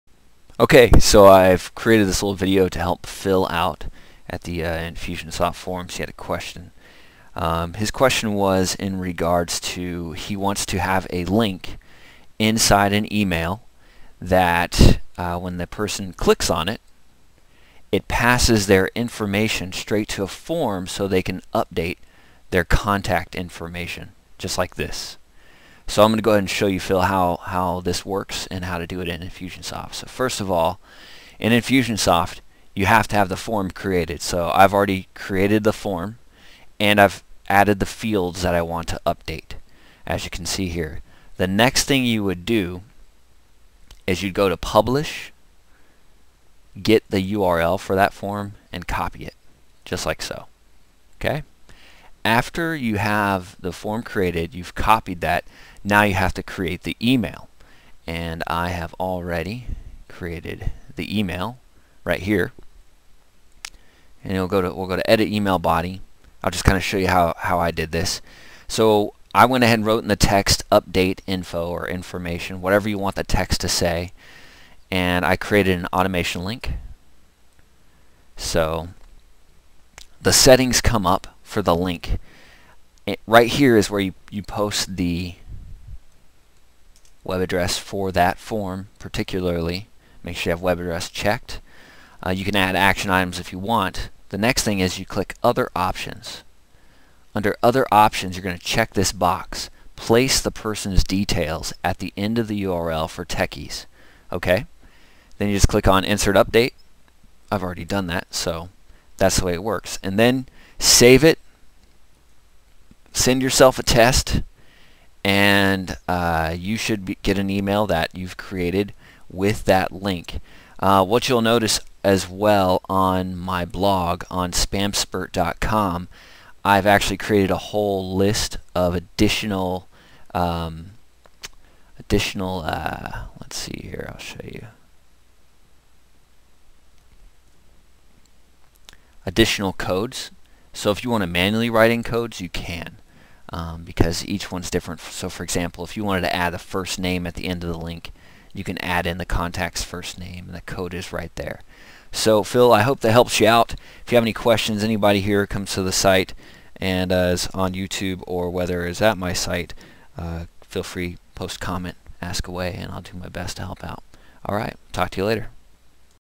Infusionsoft Update Customer Info Through a Link
This video shows you how to create a link inside an email that sends customers or prospects to a web form so they can update any and all of their contact fields.